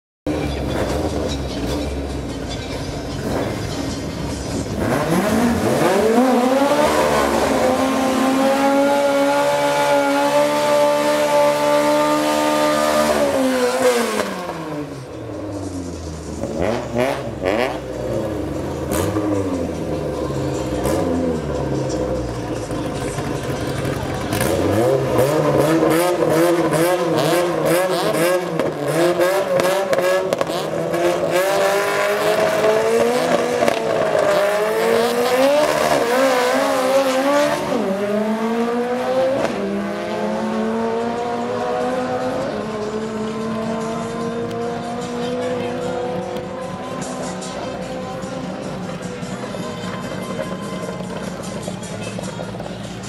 June 2015 RWUB Run 35
19th June 2015 - The Summer Night Run What You Bring event hosted by the MDRA at Hal Far Raceway